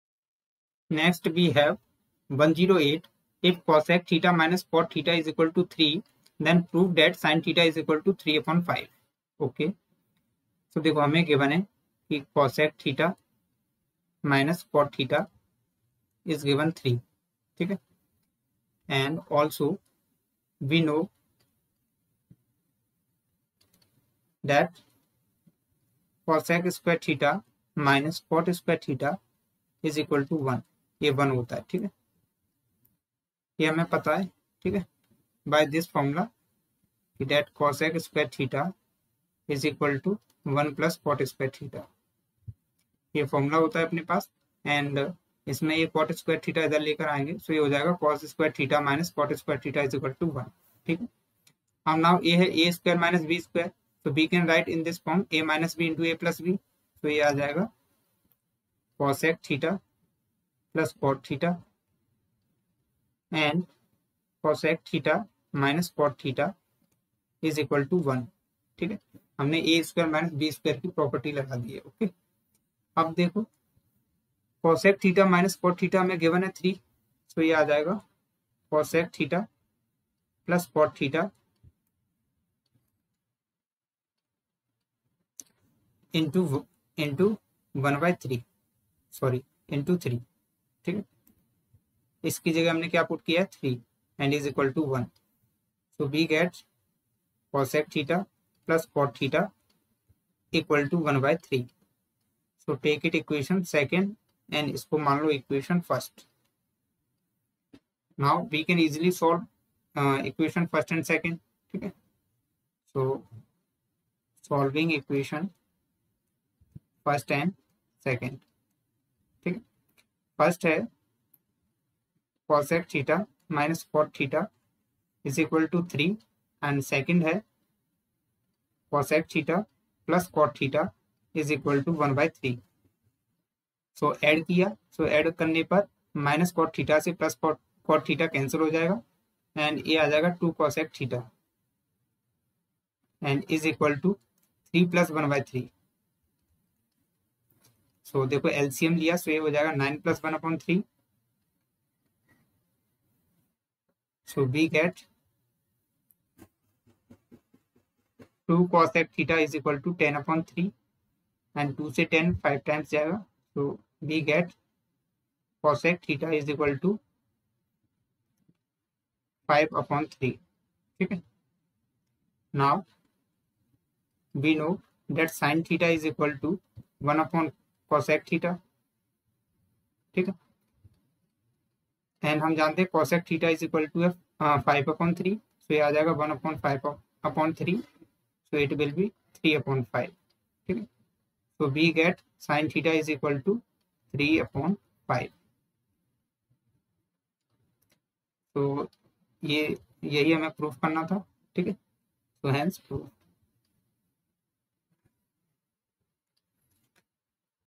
XTRIGNQ108 _ If cosec θ- cot θ=3 , then prove that sinθ= 3/5.
If cosec θ- cot θ=3 , then prove that sinθ= 3/5.      XTRIGNQ108